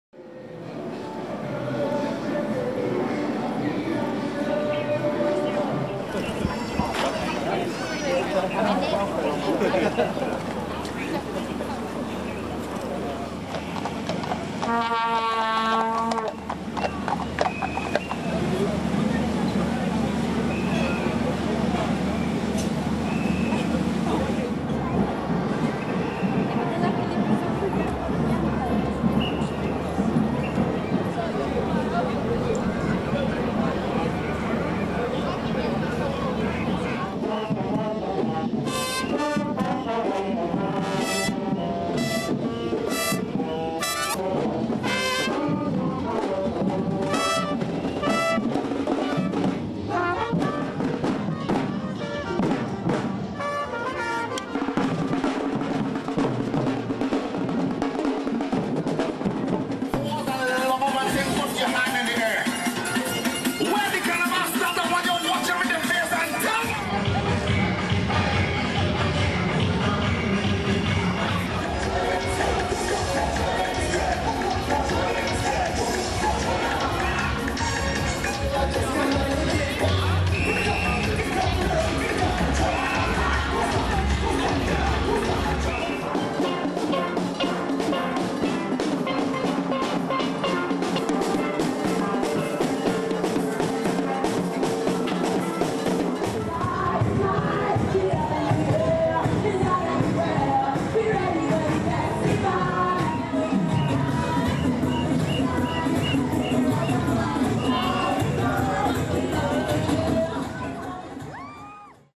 Notting Hill Carnival in London
British police were deployed on the streets of London during the Notting Hill Carnival on Sunday, mingling among Calypso dancers and steel-drum bands after intelligence suggested gangs wanted to create trouble there.